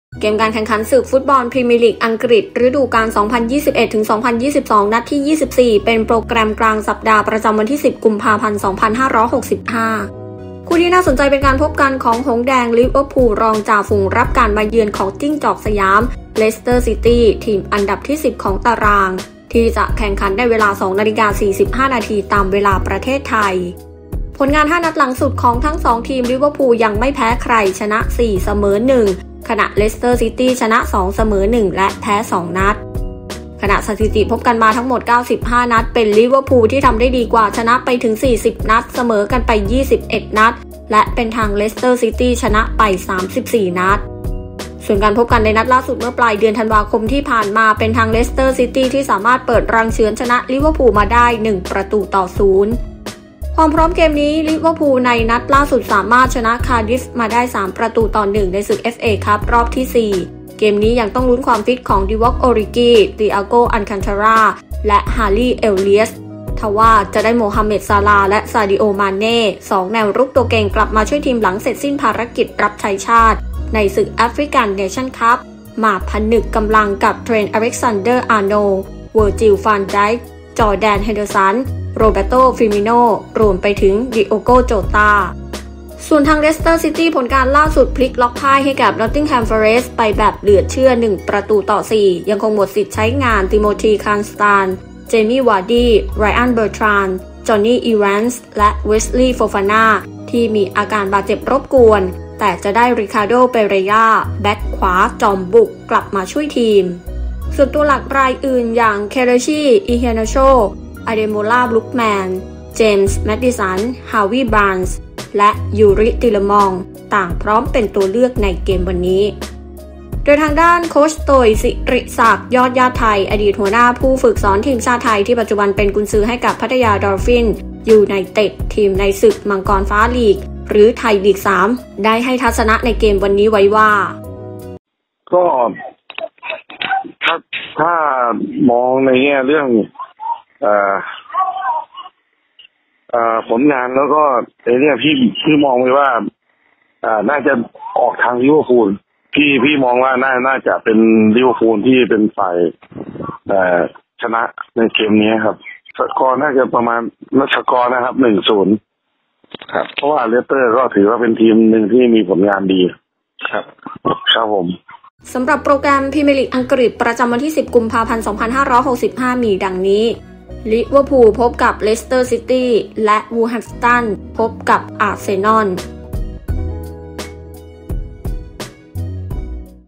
'ลิเวอร์พูล' มาเต็มพร้อมถอนแค้น 'เลสเตอร์' สบาย
เกมการแข่งขันศึกฟุตบอลพรีเมียร์ลีก อังกฤษ ฤดูกาล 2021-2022 นัดที่ 24 เป็นโปรแกรมกลางสัปดาห์ ประจำวันที่10 กุมภาพันธ์ 2565 คู่ที่น่าสนใจเป็นการพบกันของ “หงส์แดง” ลิเวอร์พูล รองจ่าฝูงรับการมาเยือนของ “จิ้งจอกสยาม” เลสเตอร์ ซิตี้ ทีมอันดับที่ 10ของตาราง ที่จะแข่งขันในเวลา 02.45 น. ตามเวลาประเทศไทย